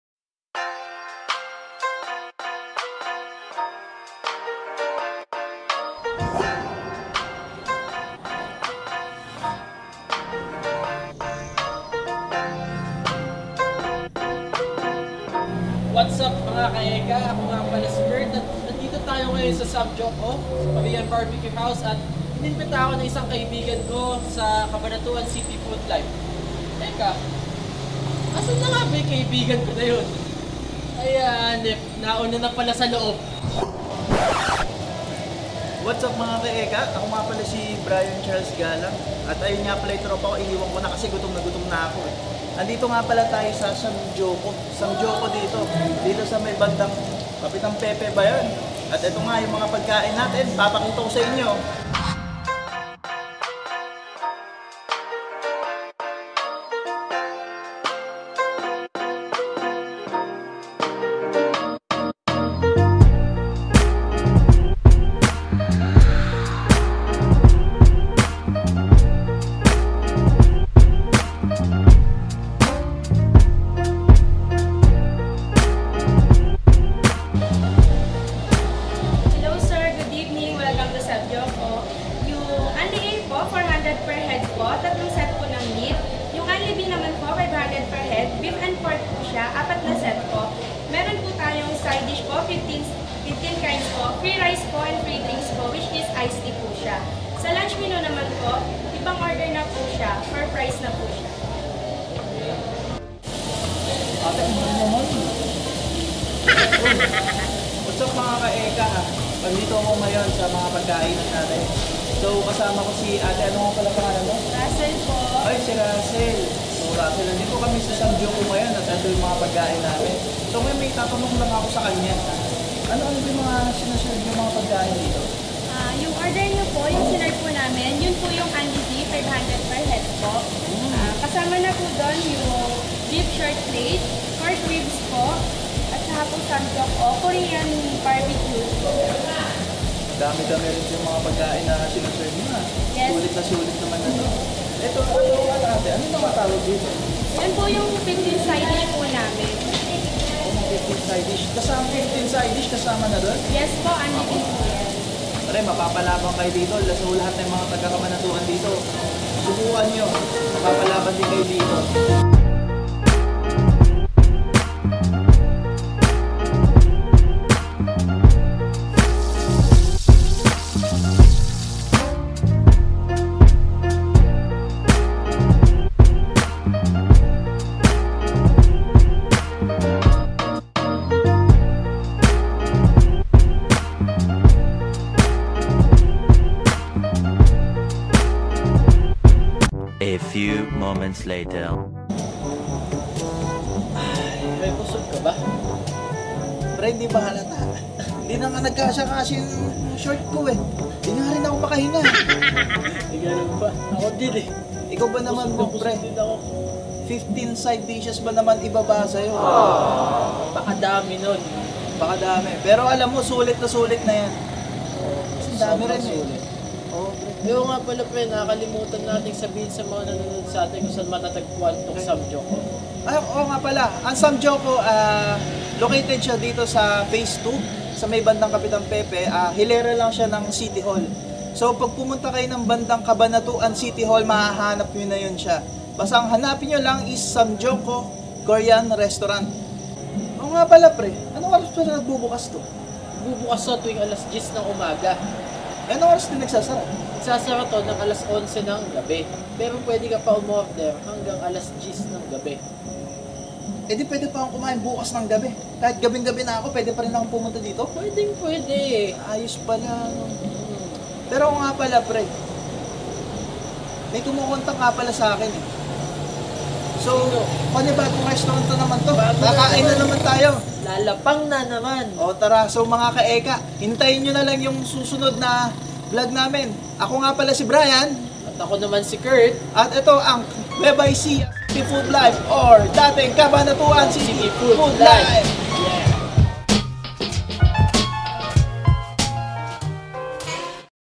Samjok-O Korean Restaurant | NE FOOD LIFE VLOG
mga ka eka! siguraduhin niyo muna na magpakabusog kayo bago kayo manood neto! eka nga kasi nila ee magugutom ka at matatakam. HAHAHAHA kasama natin sa ang food vlogger na si Kurt Cheng at ang blogger owner na si Brian Galang!